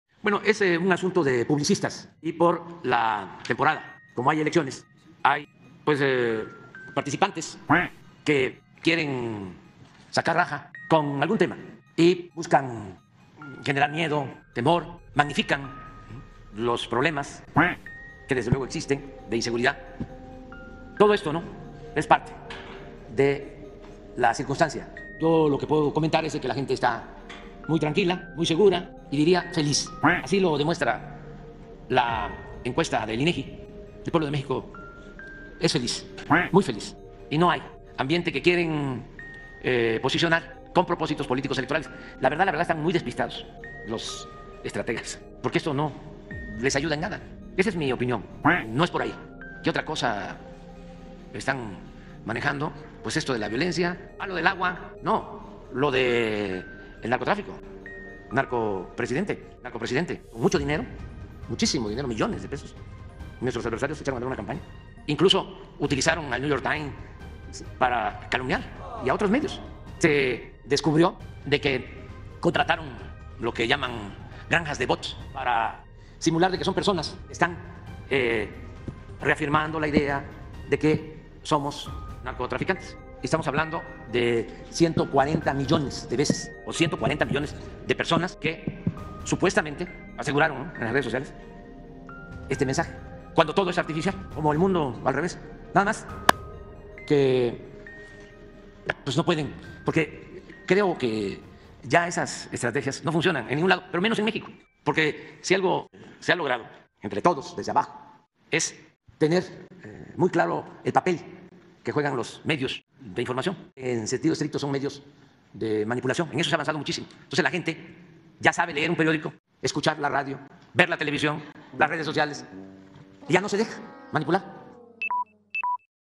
La violencia en el proceso electoral “es un asunto de publicistas, la gente está muy feliz”: AMLO® 🦆
La violencia en el proceso electoral “es un asunto de publicistas”, “magnifican los eventos de inseguridad”, “la gente está muy feliz”: AMLO® 🦆